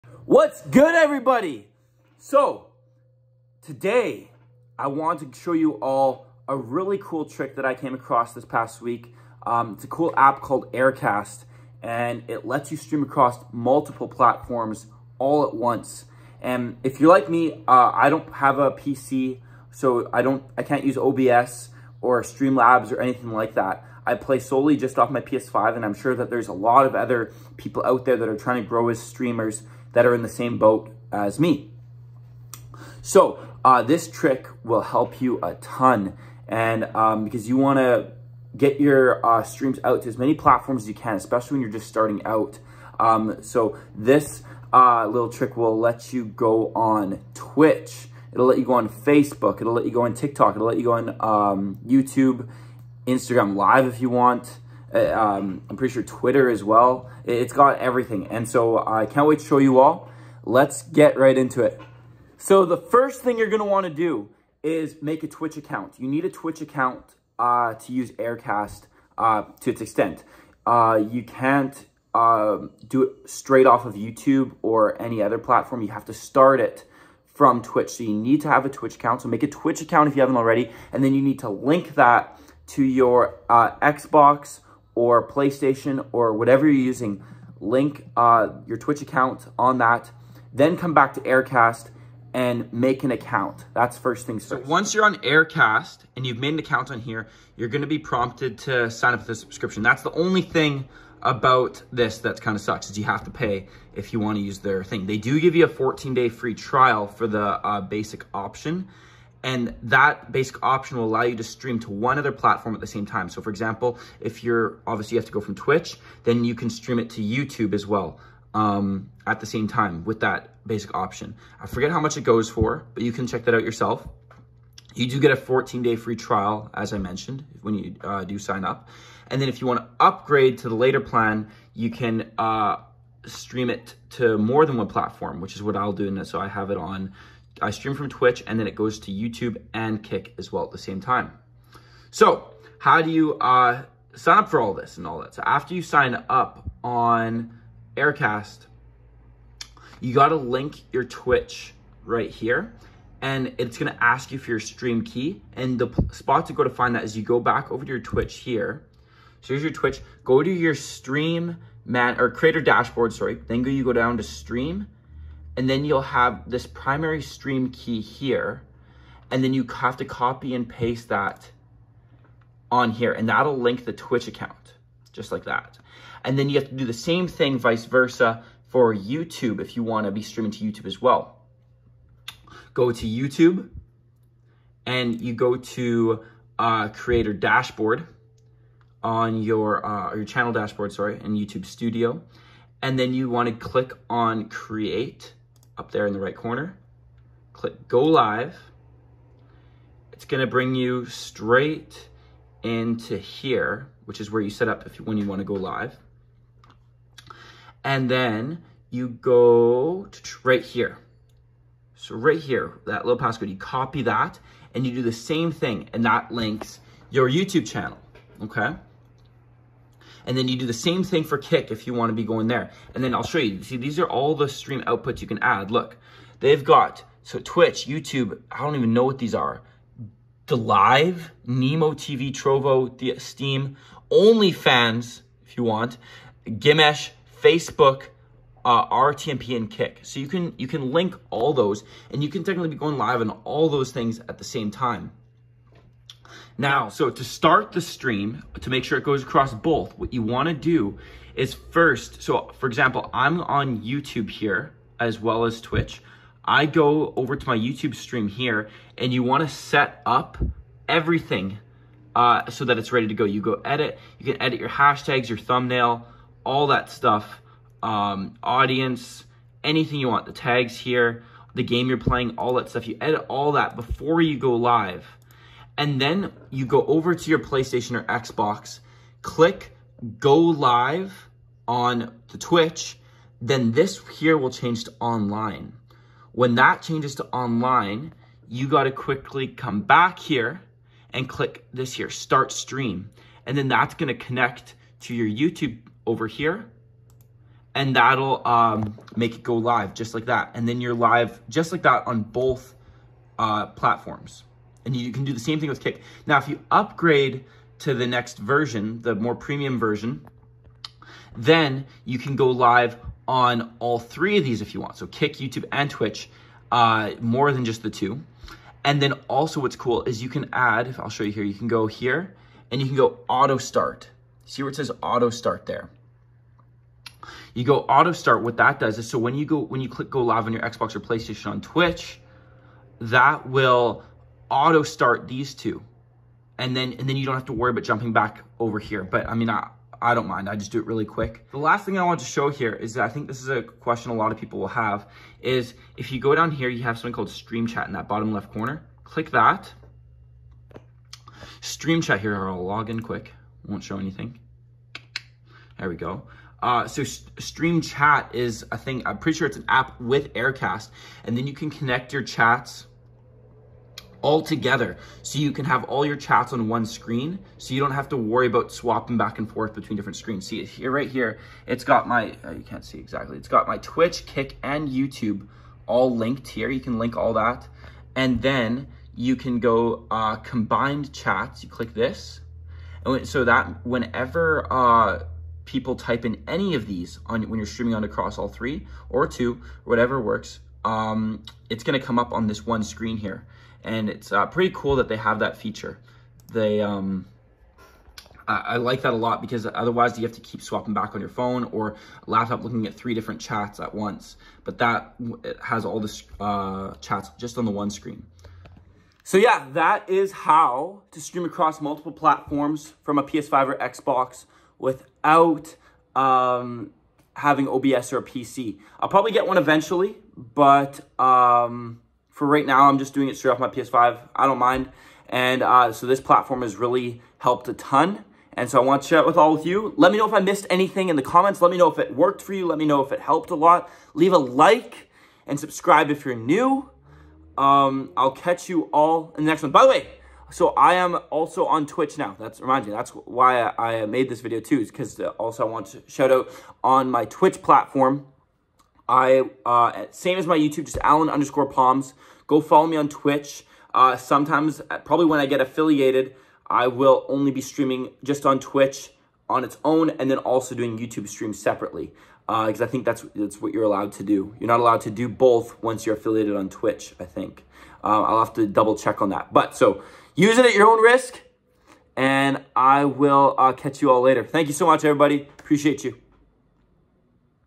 How to stream on Kick, Twitch & YouTube all at once, from a PS5 (without OBS)
In this video, I go over how to aream on multiple platforms at once without OBS OBS or Streamlabs

I go live almost every night at 10:30pm CST!